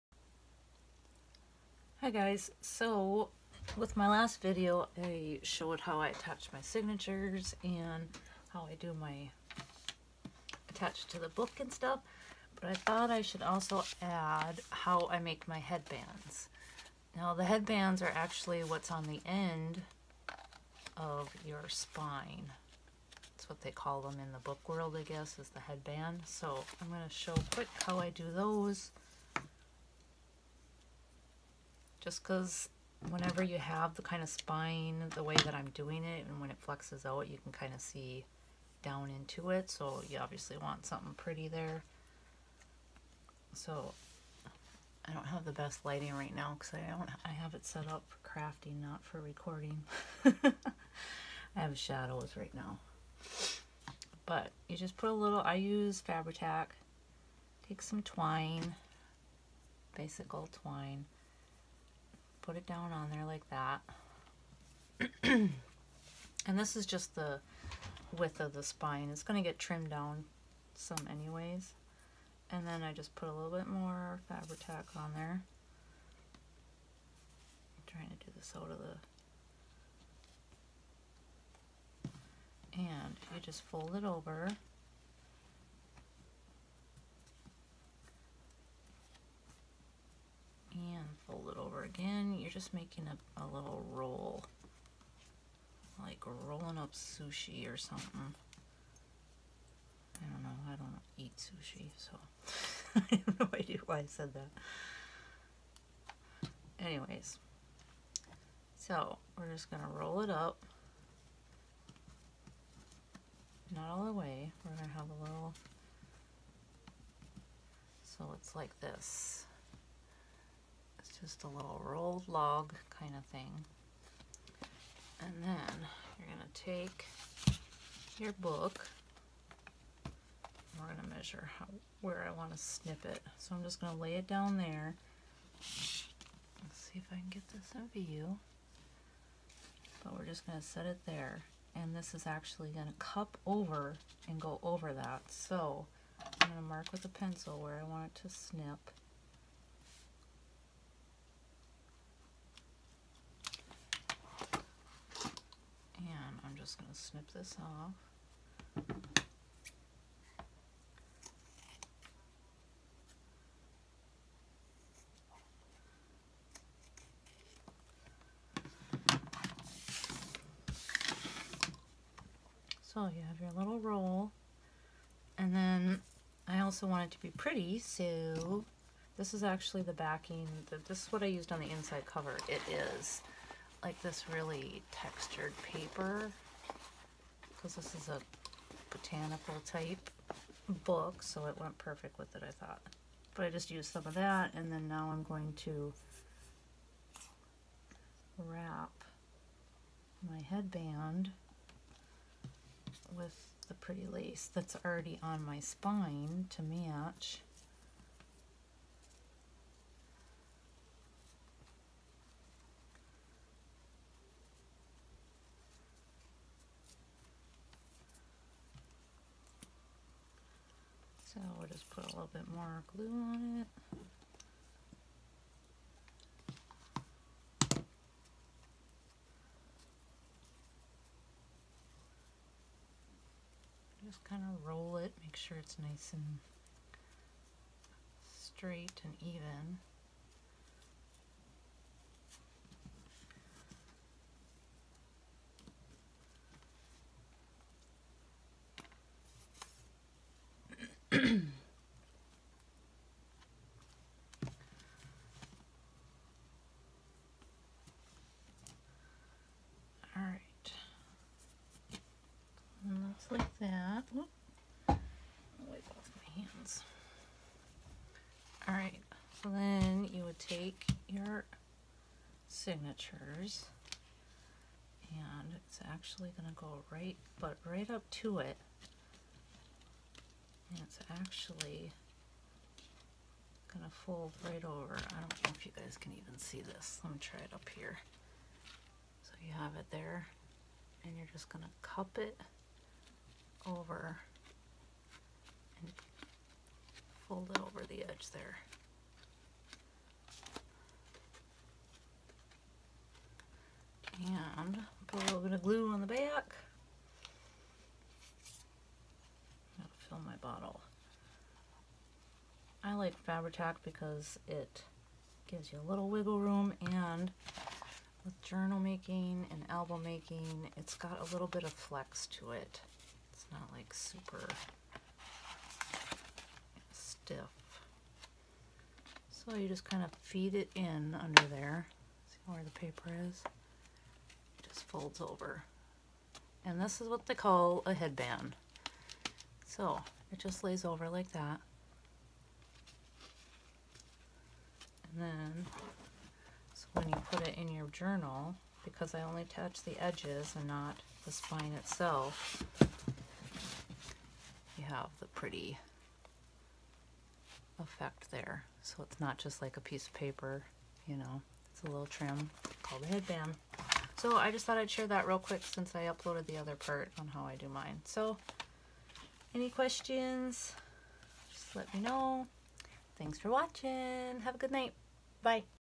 Making and attaching “headbands” to your spine - tutorial
Quick little tutorial on making and attaching headbands to your spine in journal making. Hope this helps a little.